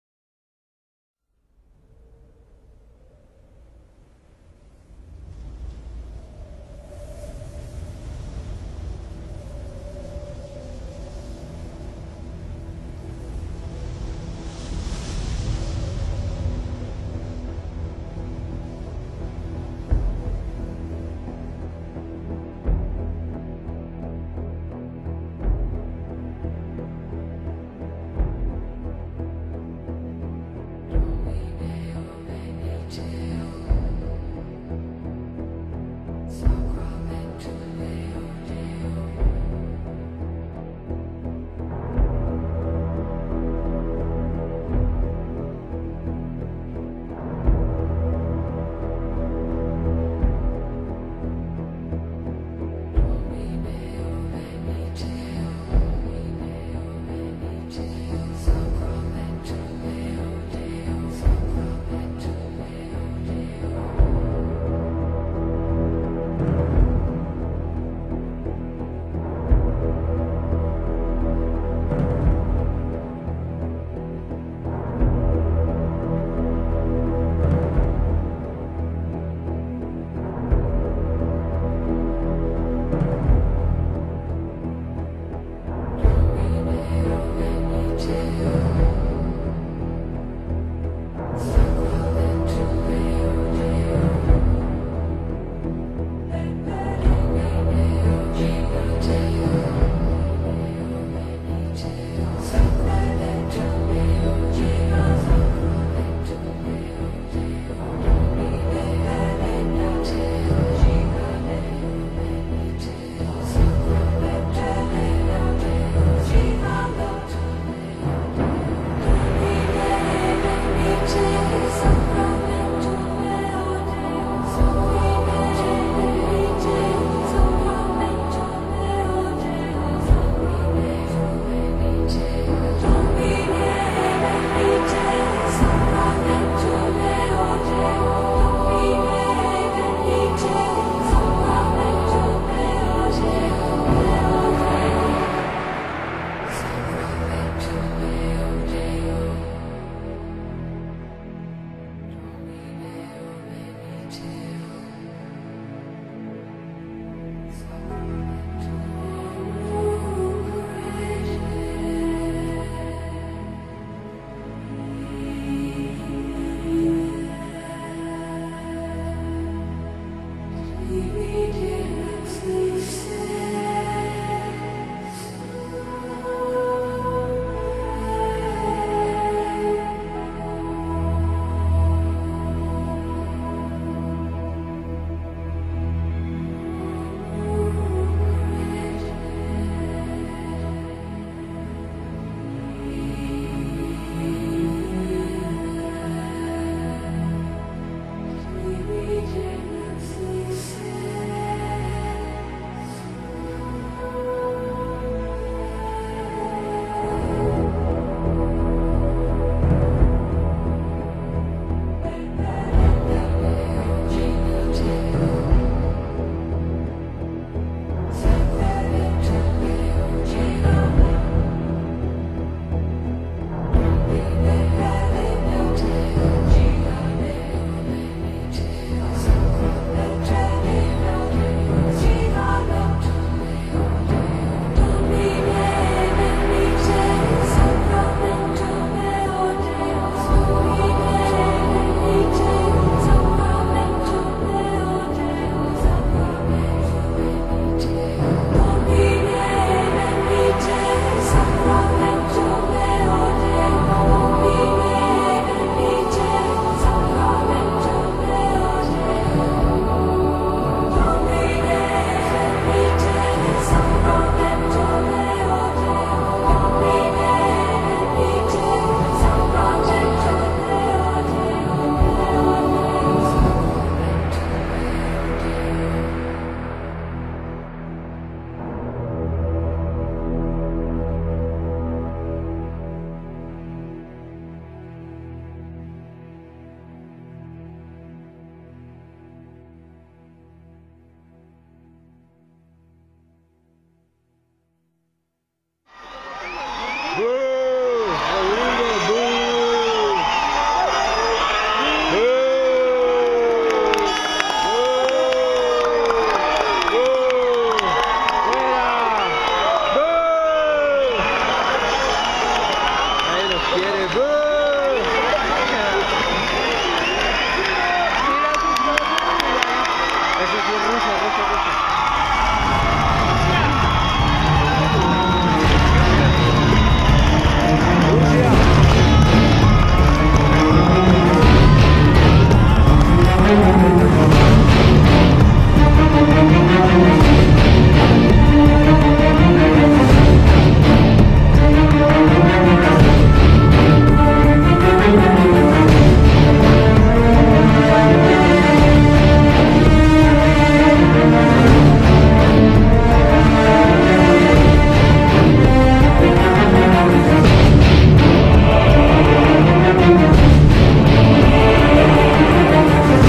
Mexican Special Forces - Desfile de las Fuerzas Especiales de Mexico Russia Y China
Este es el desfile del bicentenario de las fuerzas especiales de mexico con la participacion de las escoltas de rusia y china

Russian and Chinese Troops Marched in Parade in Mexico

Nada de este material es mio solo lo hice con proposito de entretenimiento sin tratar de ofender a nadien